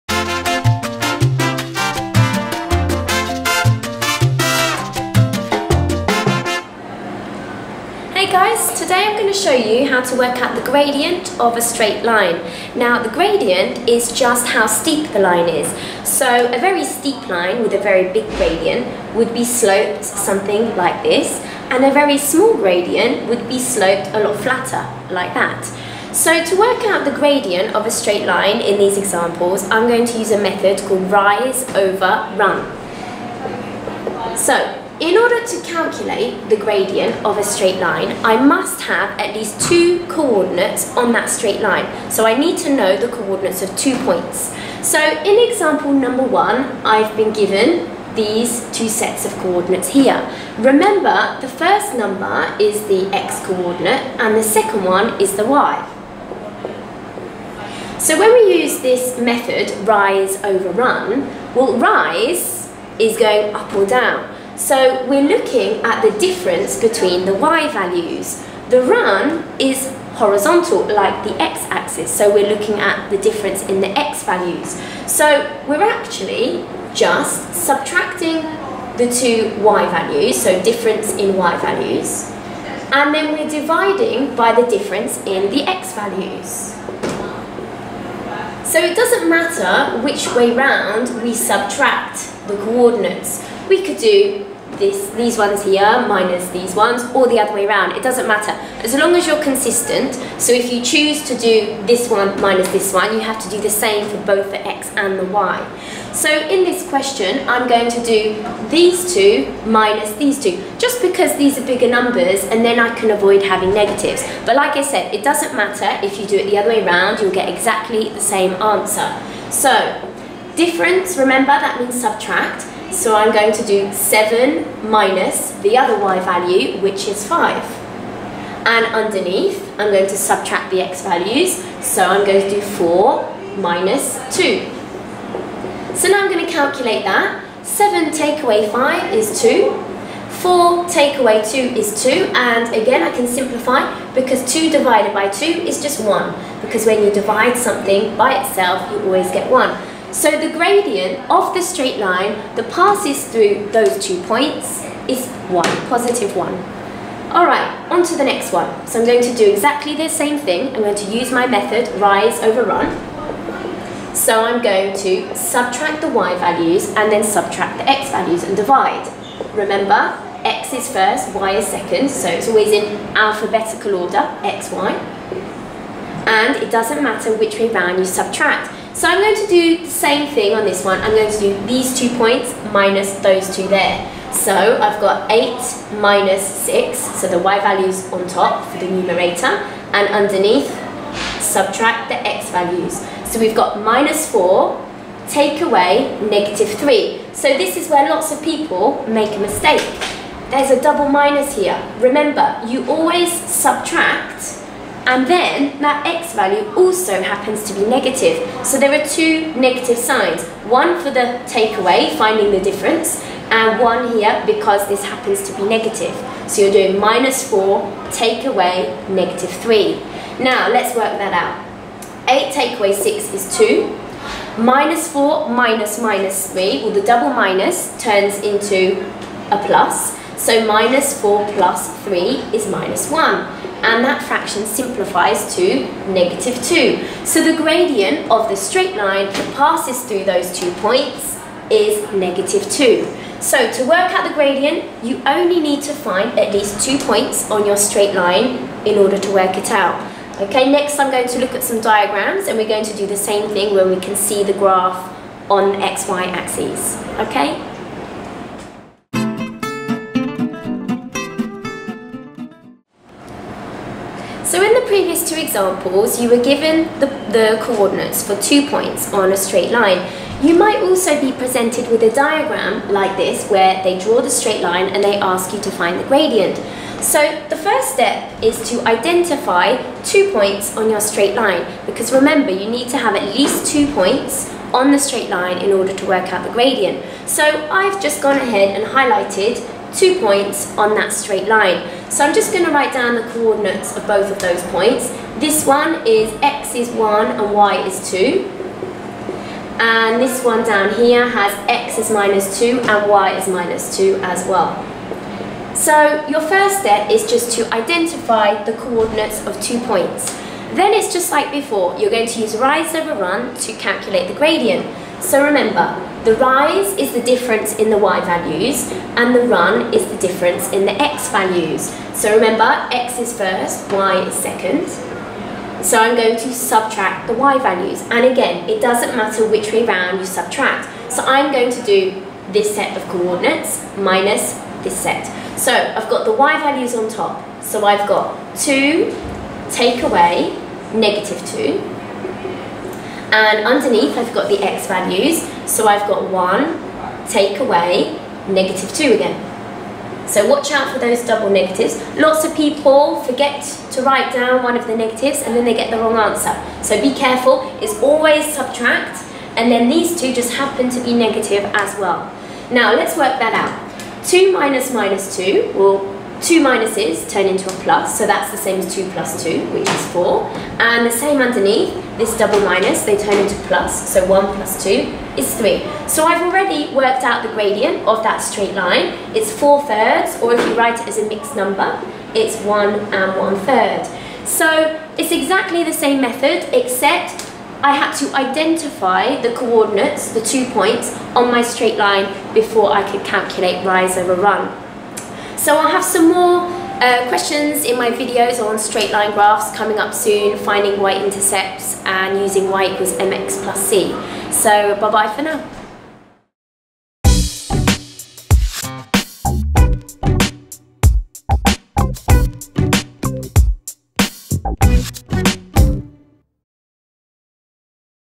The Maths Prof: Finding Gradients of Straight Lines (using rise over run)
In this lesson I show you how to work out the gradient when you are given either two points on the line or a sketch of the line. If you want to find the equation of a straight line watch this lesson